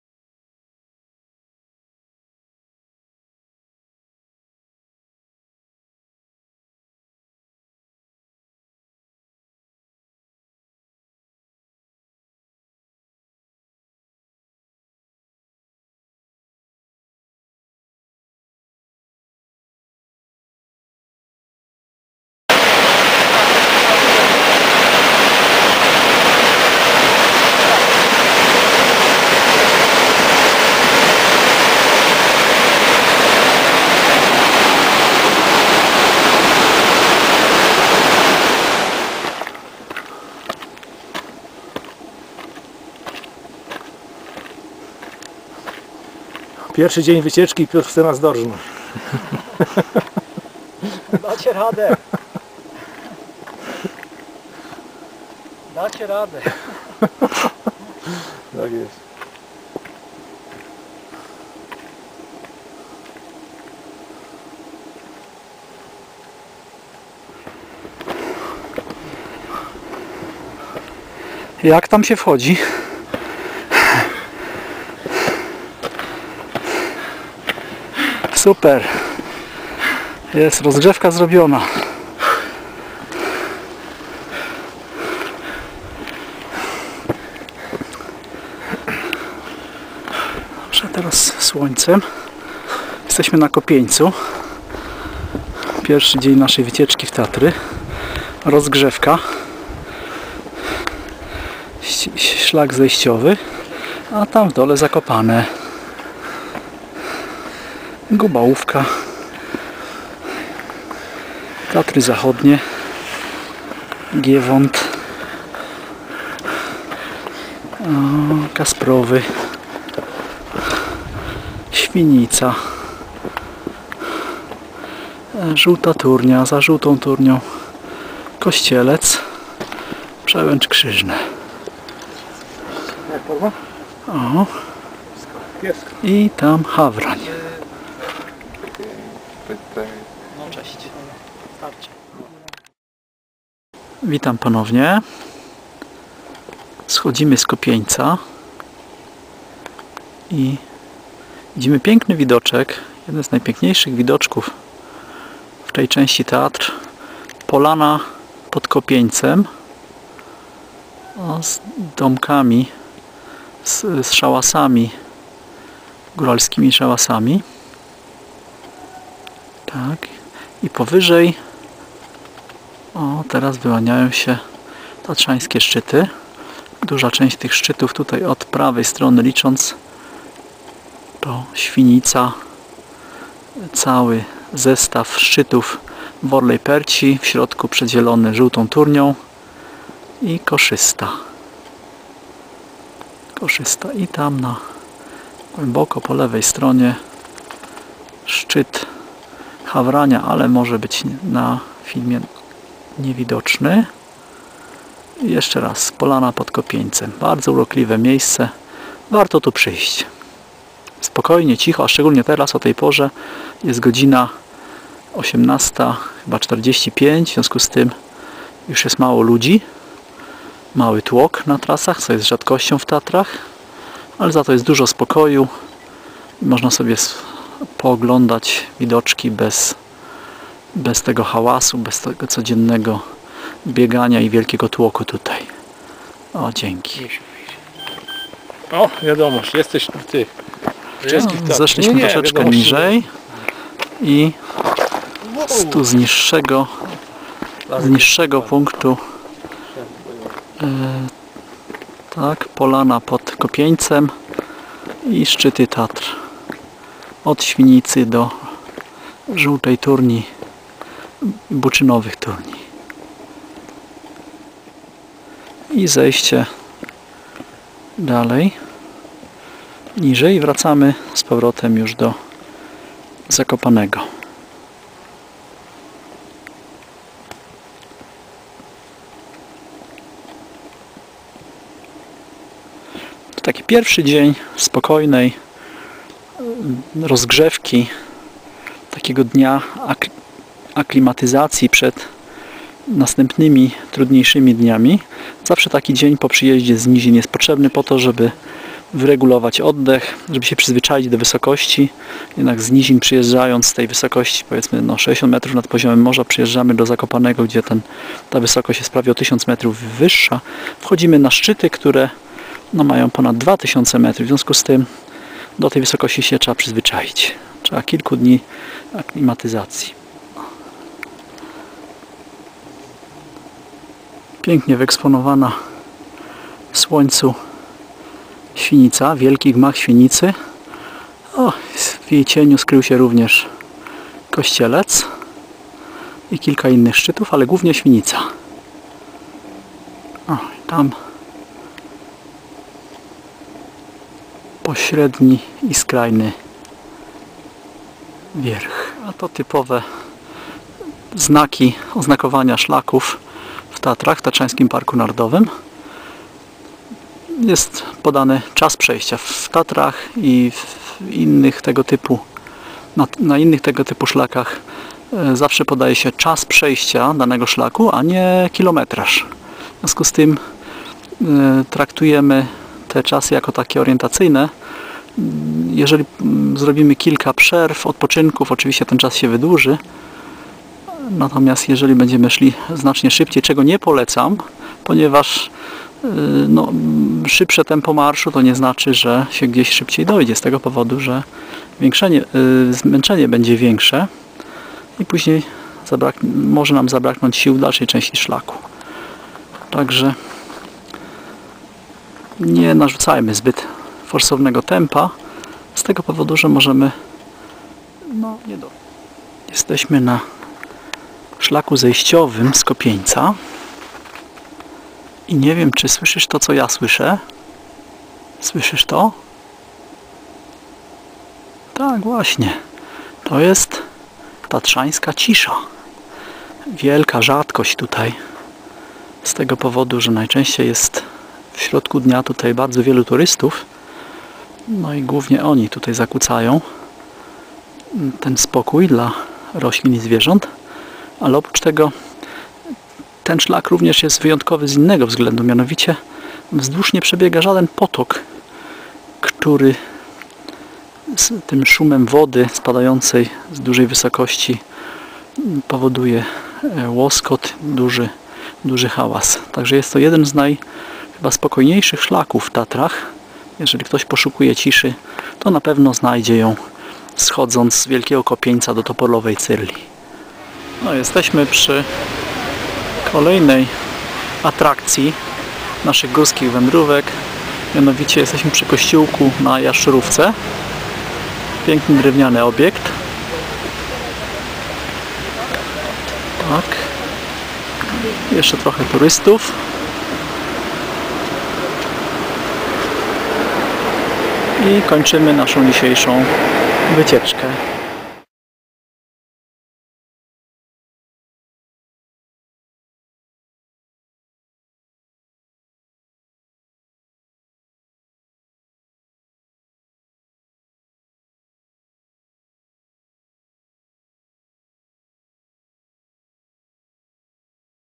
Tatry. Kopieniec
Film z komentarzem o wartości górskich wędrówek w zdrowym stylu życia